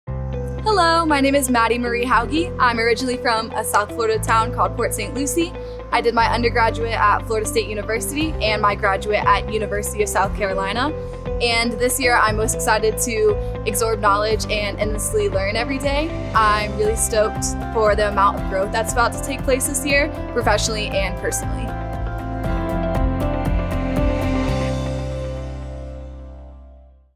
Athletic Training Resident Maddy Hauge
The Steadman Clinic welcomes Maddy Hauge, one of our Athletic Training Residents for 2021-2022!